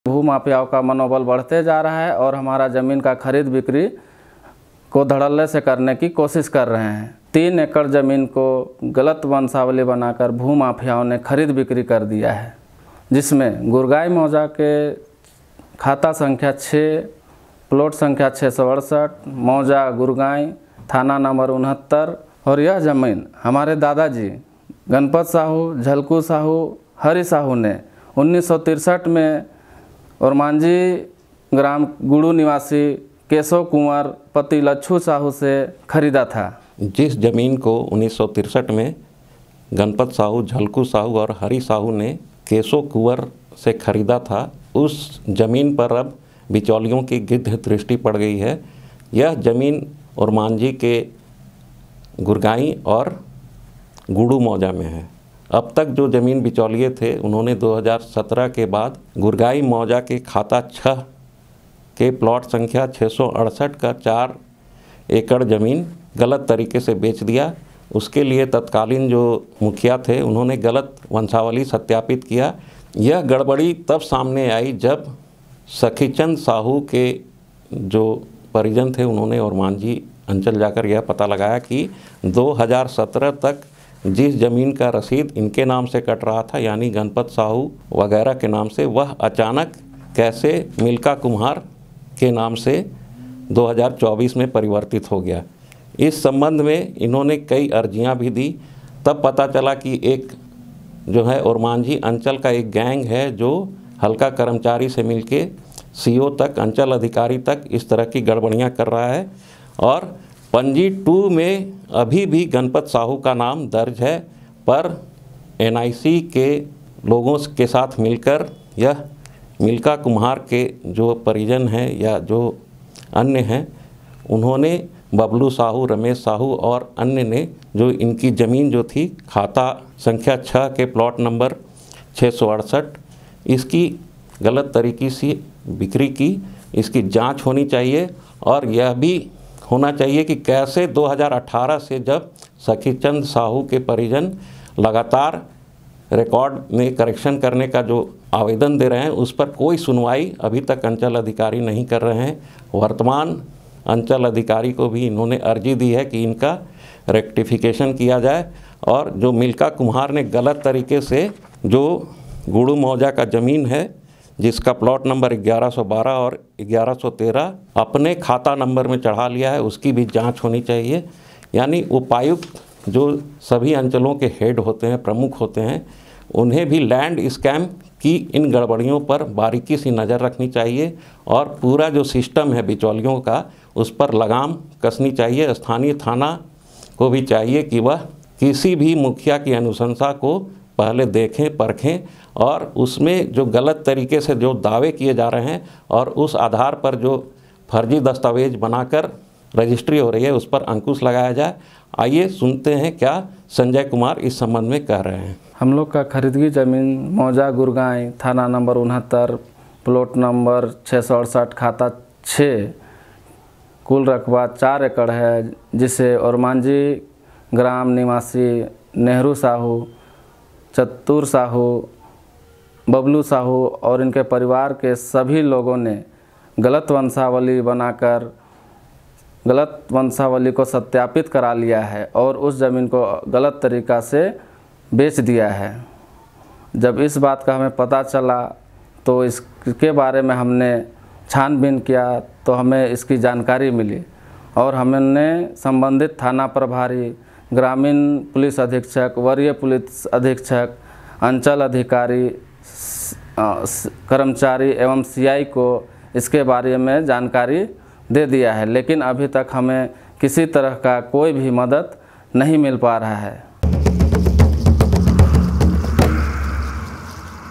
LOKTANTRA19 : ओरमांझी अंचल के कर्मियों की मिलीभागत से मिल्का कुम्हार के परिजनों ने गणपत साहू की...
ओरमांझी अंचल के कर्मियों की मिलीभागत से मिल्का
कुम्हार के परिजनों ने गणपत साहू की जमीन बेची